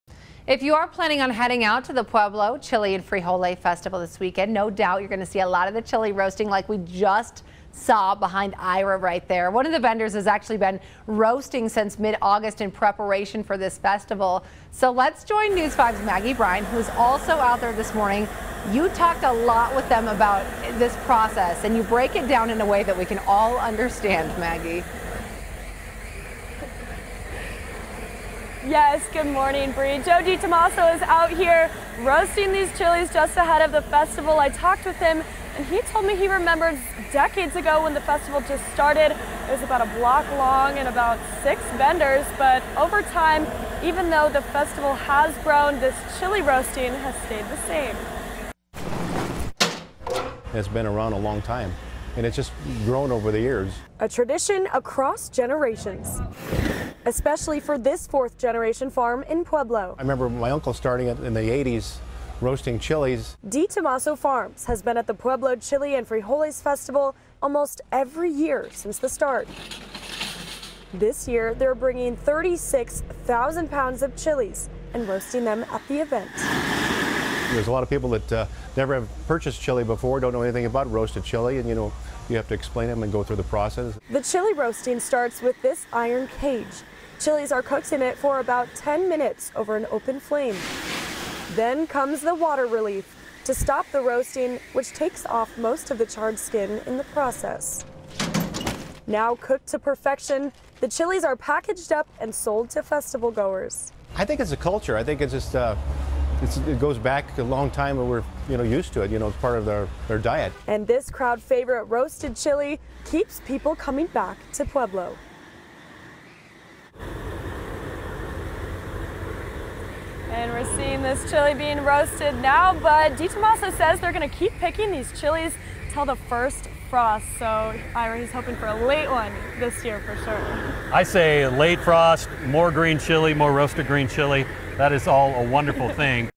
Chile and Frijole festival kicks off in Pueblo
If you're planning on heading out to the Pueblo Chile and Frijole festival this weekend, no doubt you'll see a lot of chile roasting. One of the vendors has been roasting since mid-August in preparation for the festival. News5's Maggie Bryan spoke with them about the process.